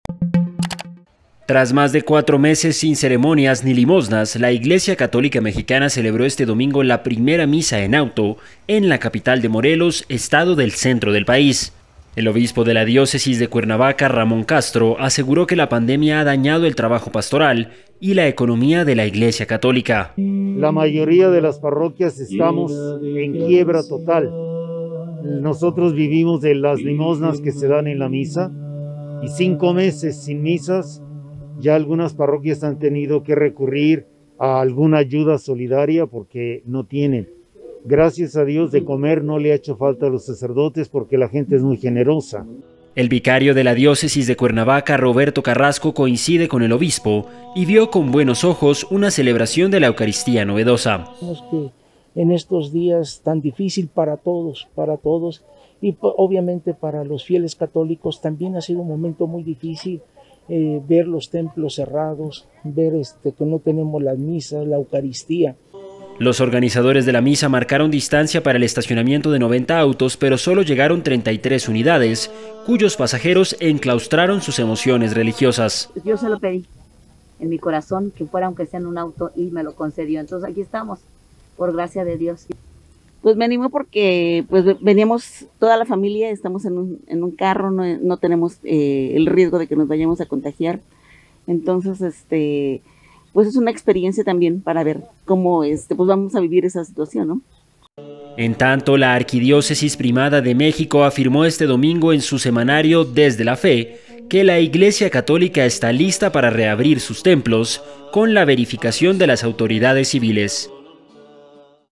Ciudadanos de Morelos celebran la primera misa católica en auto en México
Tras más de cuatro meses sin ceremonias ni limosnas, la Iglesia Católica mexicana celebró este domingo la primera misa en auto en la capital de Morelos, estado del centro del país.
¡Visítenos!